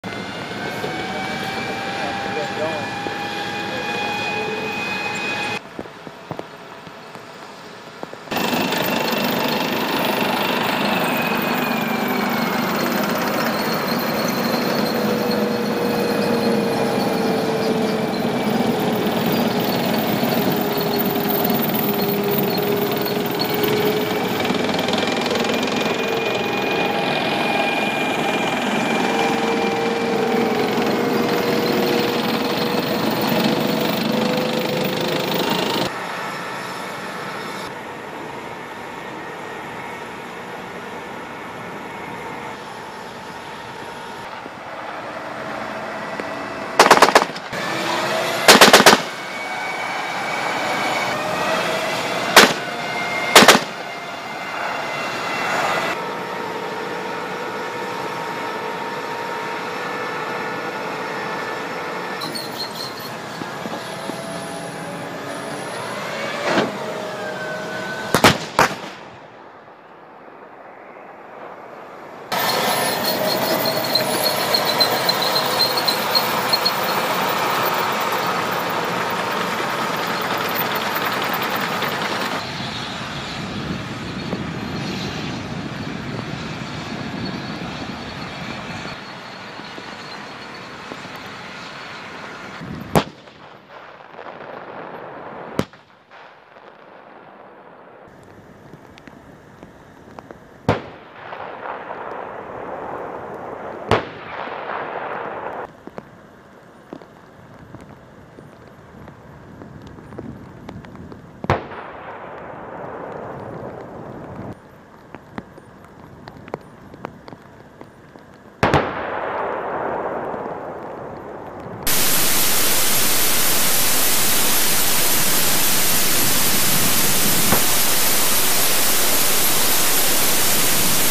US Military M1 Abrams Tank - Awesome Gunnery Range
Soldiers from 1st Brigade Combat Team, 1st Cavalry Division conduct gunnery training with M1 Abrams tanks at Grafenwoehr Training Area (GTA), Germany as part of the multinational training exercise Combine Resolve III.

Video by Matthew Oldham | 7th Army Joint Multinational Training Command | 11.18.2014